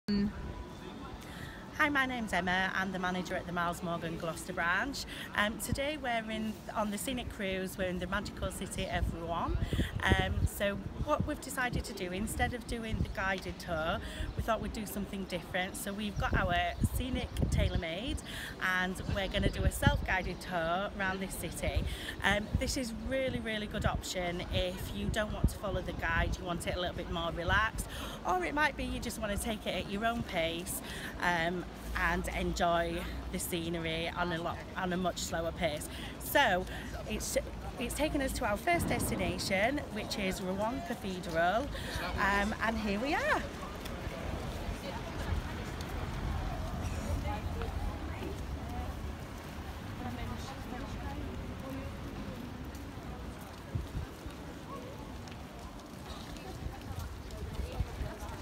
Emma - Scenic River Cruise Excursion - Rouen Cathedral
Follow Emma O'Donovan, Manager at our Gloucester branch as she shares her excursion experience with Scenic.

To browse our selection of stunning Scenic River Cruises go

To browse our range of cruise and holiday destinations go